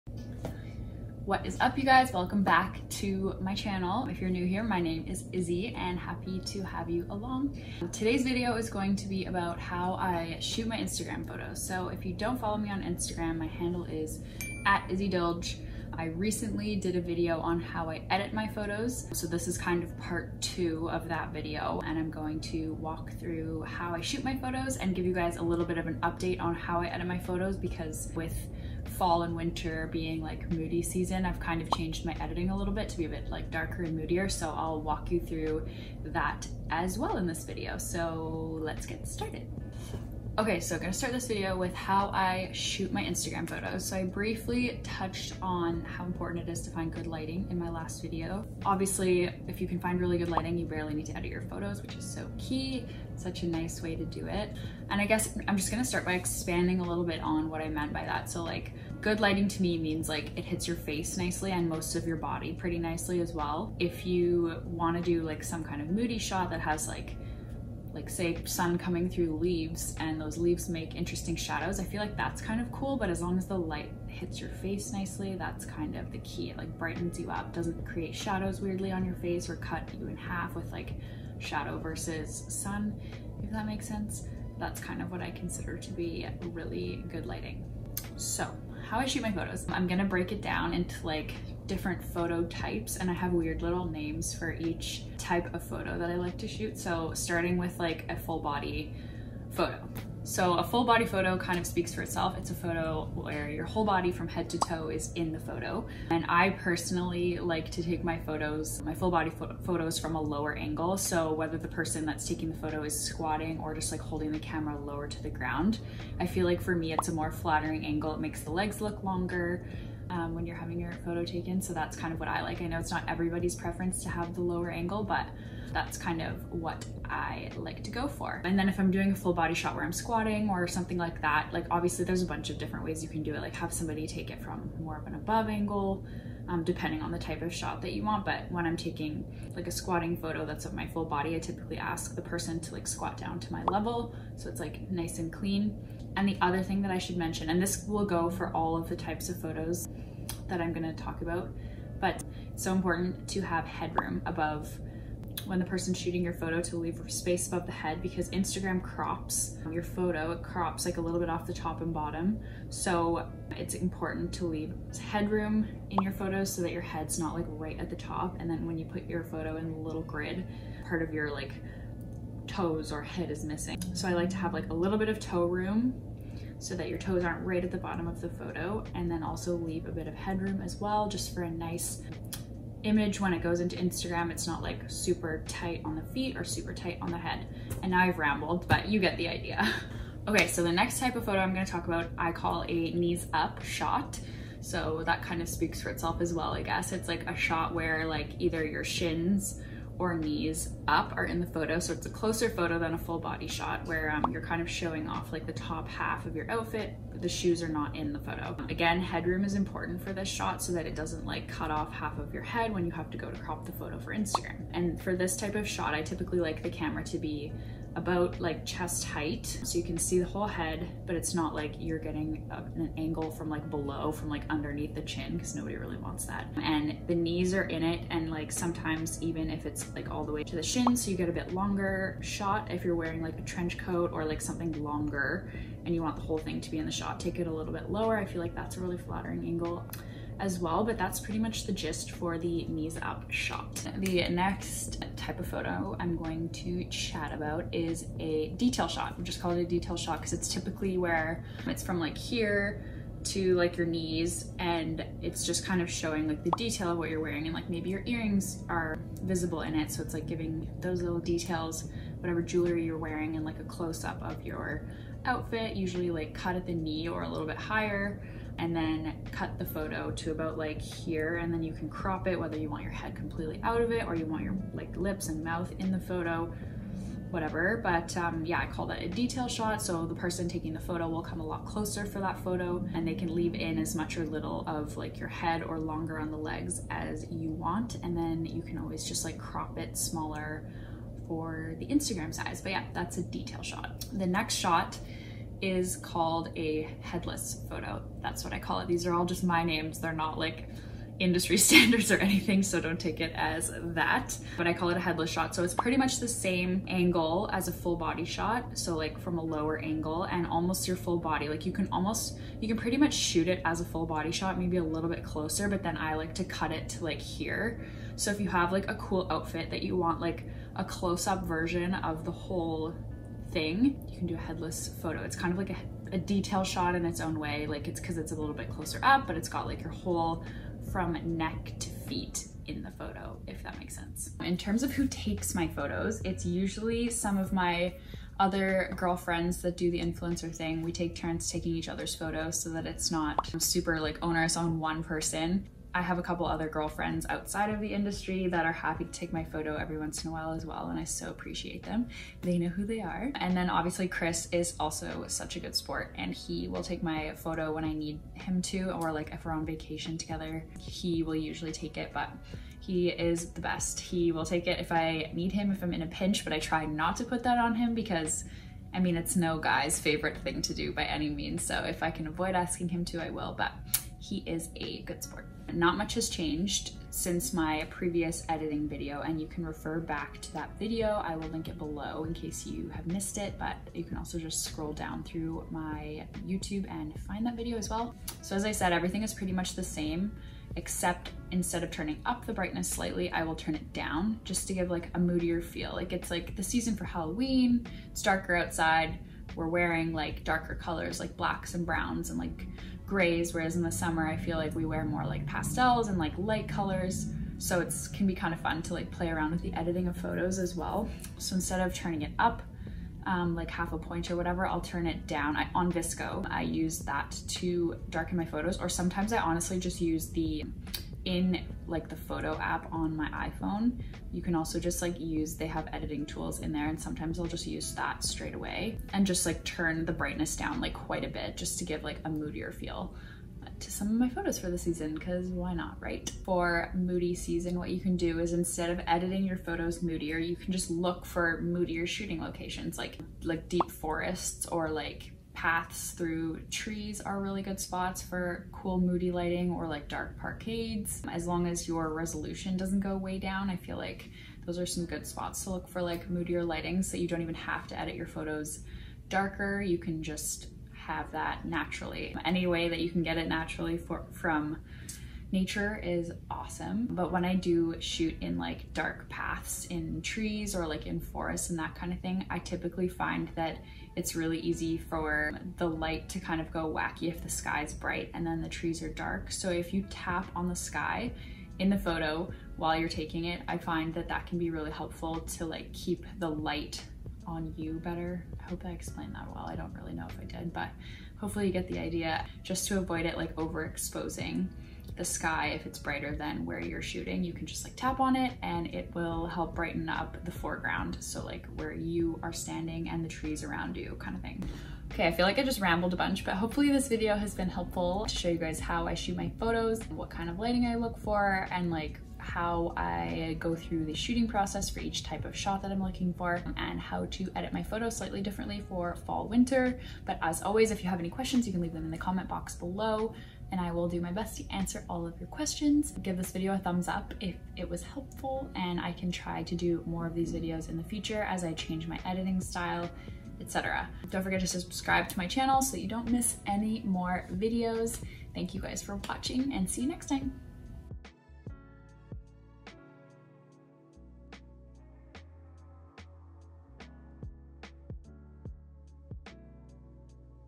HOW I SHOOT MY IG PHOTOS: Tips for Shooting Instagram Content, Best Way to Take Instagram Photos
Hi Babes!

If you are new around here, I'm Izzy and I'm a fashion blogger from Vancouver!

As promised, I'm sharing this video on how I shoot my IG photos and content creation strategy - including how to pose for photos, best angles and (in my opinion) the best way to take Instagram pictures.

If you are looking to grow your Instagram and become a content creator/influencer in 2020 then this video has all the tips and tricks for you! I dive deep into it all including how I take and edit my Instagram photos for this fall and winter, lighting hacks and things you should be telling your photographer to do. Follow these simple tips and learn how to become an Instagrammer and grow you Instagram in 2020.

Edited by Jacci Rai (@jaccirai)